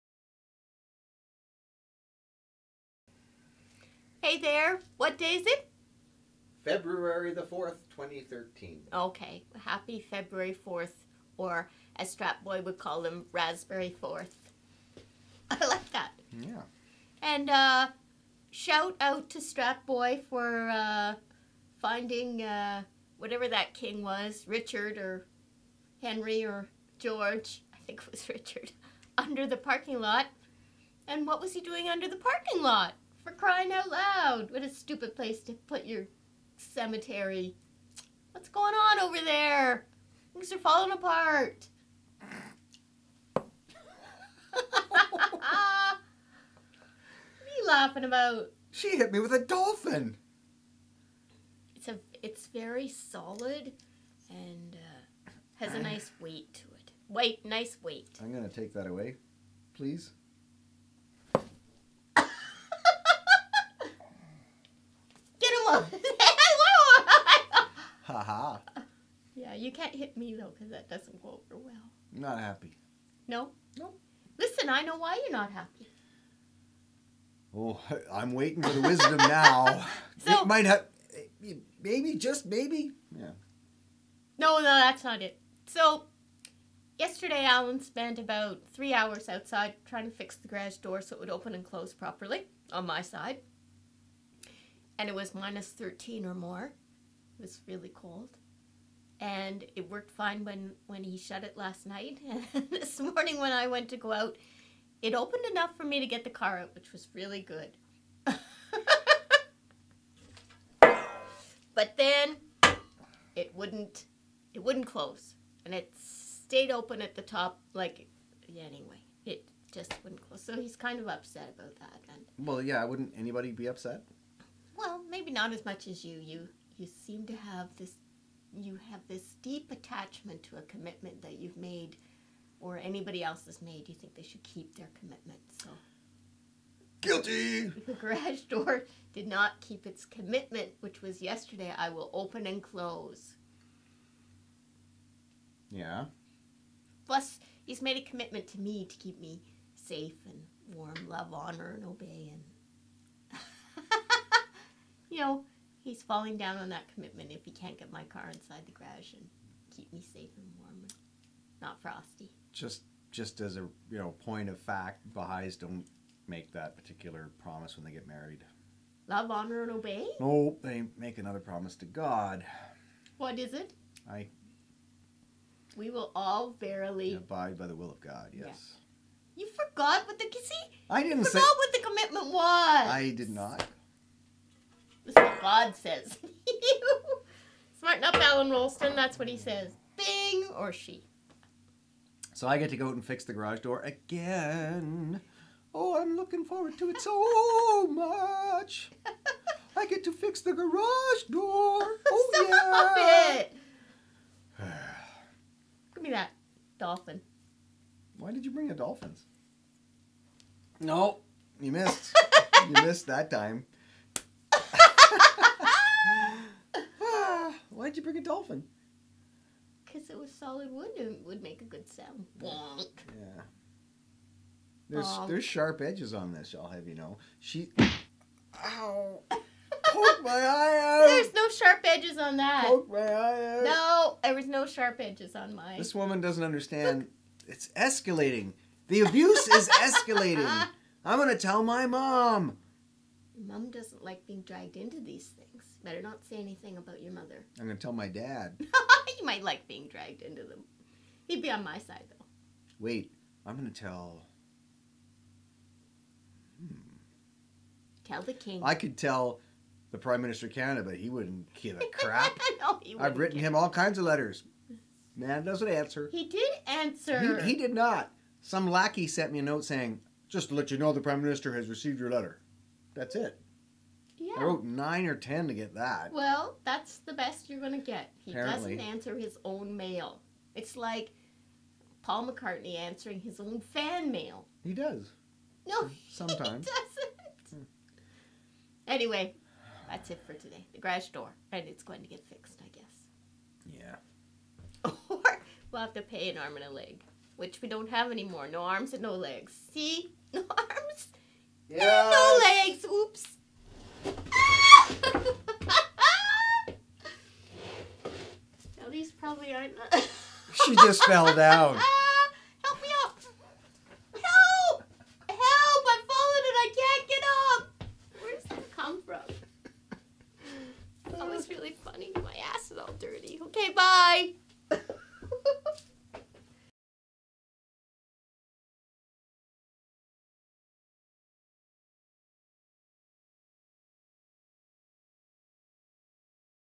Garage Door #2
Jo noticed Allen isn't in a great mood. Allen thinks it has something to do with being hit on the head with a wooden dolphin.  Jo thinks it has something to do with Allen's attachment to keeping commitments.
maxtrack VBlog 04 February 2013